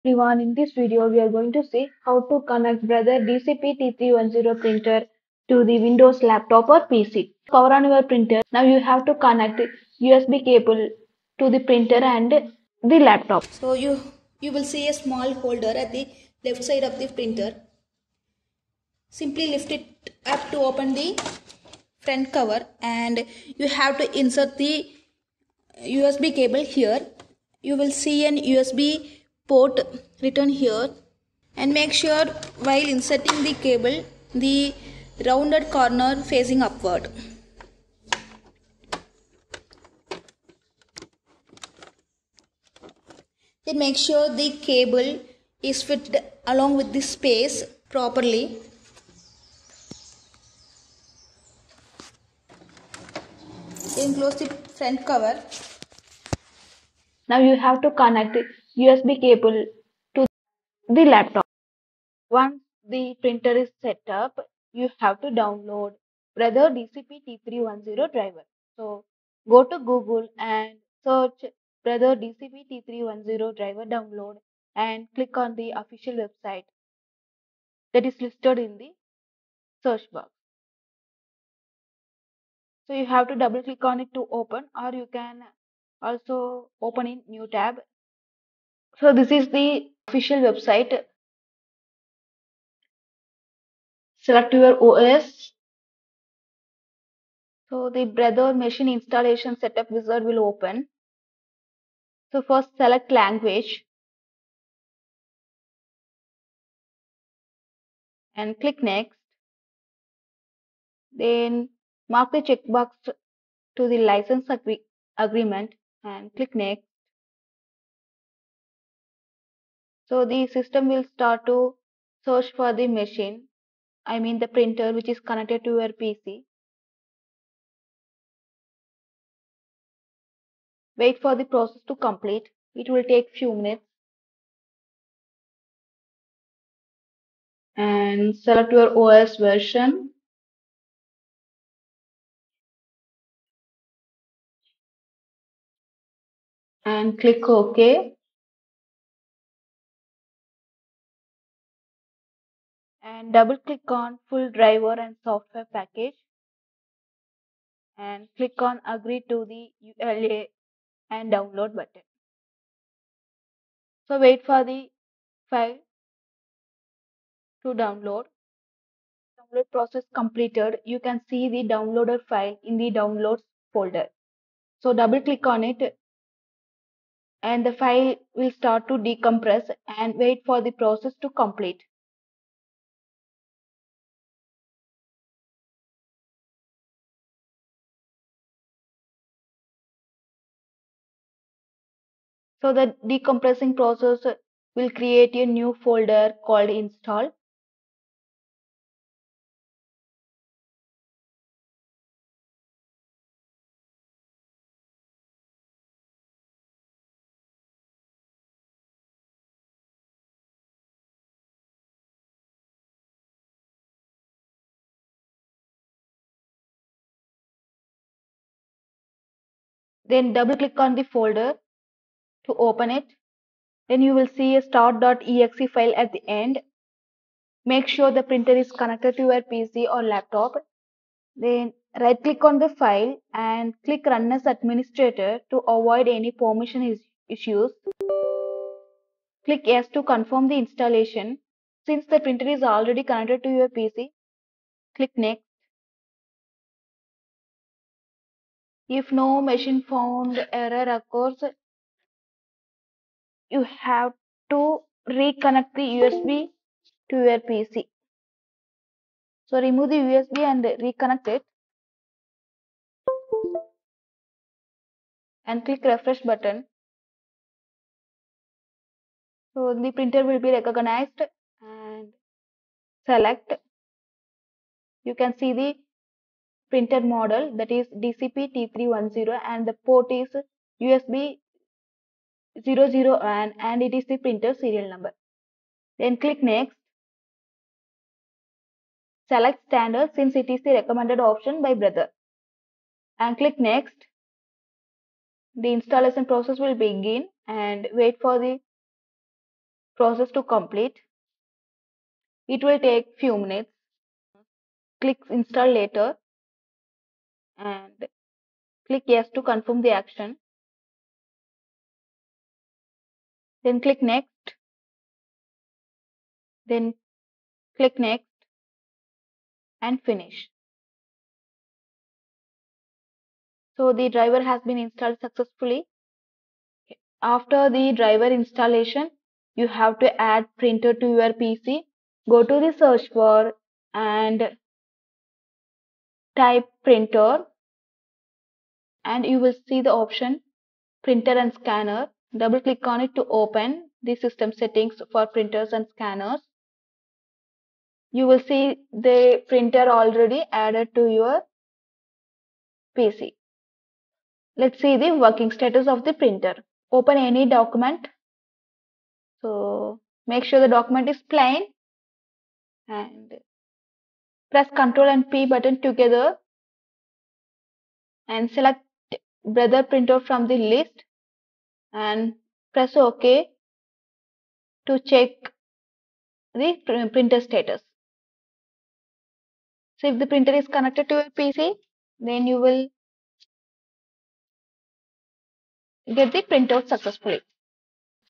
Brother DCP T310 Printer: How To Connect Laptop Windows PC
In this informative video, we provide a step-by-step guide on how to connect your Brother DCP T310 printer to a Windows laptop or PC. Whether you are setting up your printer for the first time or troubleshooting connection issues, this tutorial will walk you through the necessary procedures to ensure a successful connection. Join us as we explore the essential settings and tips for optimal performance.

Step 1: 00:07 Connect Printer To Laptop
Step 2: 01:03 Download Printer Driver
Step 3: 02:50 Decompressing The Downloaded File
Step 4: 03:42 Install The Brother Software
Step 5: 05:30 Add Printer To your PC